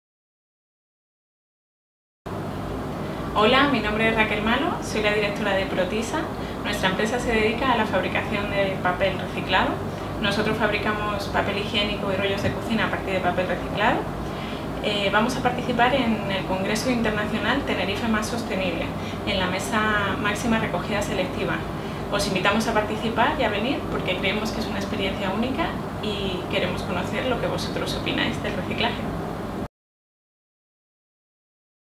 Sra. Dña. Raquel Malo Serisa - Protisa
Dña. Raquel Malo Serisa, Directora de Protisa estará presente en el I Congreso Internacional Tenerife + Sostenible organizado por el Cabildo de Tenerife, los días 9 y 10 de junio en el Auditorio de Tenerife.